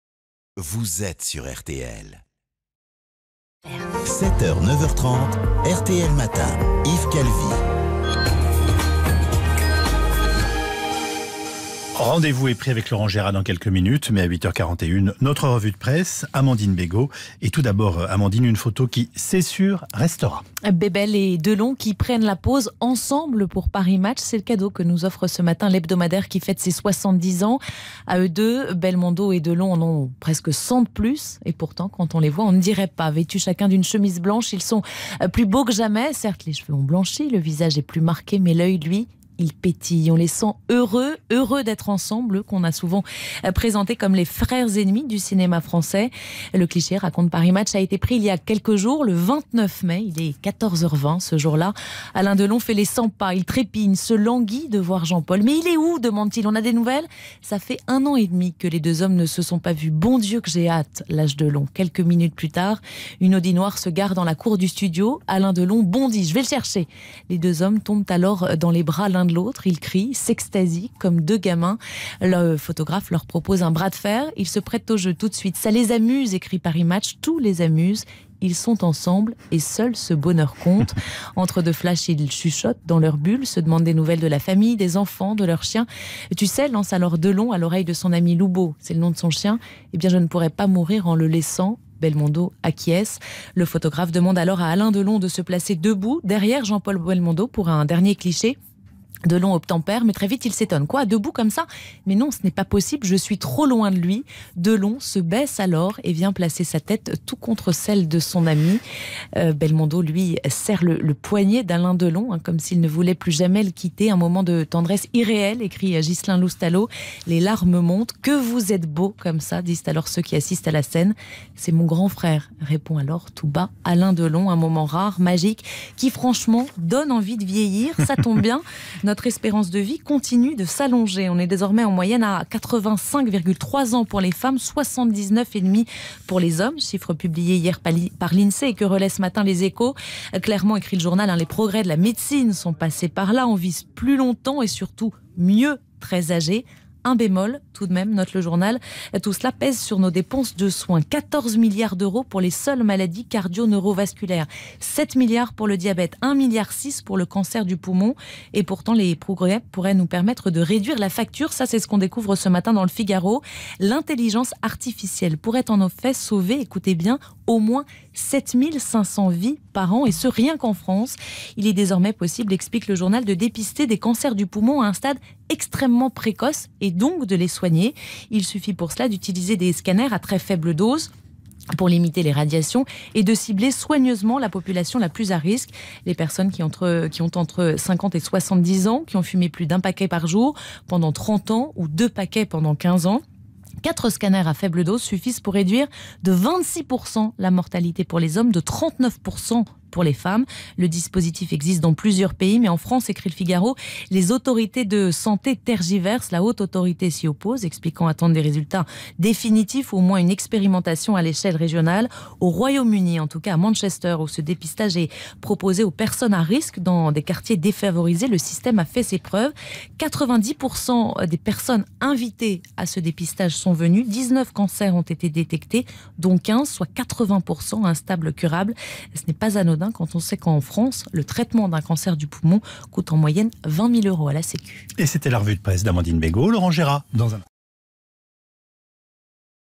Et si l'intelligence artificielle pouvait sauver des vies ?
L'espérance de vie qui augmente a également pour conséquence de faire augmenter les dépenses de santé. Mais l'intelligence artificielle pourrait bientôt être amenée à sauver des vies.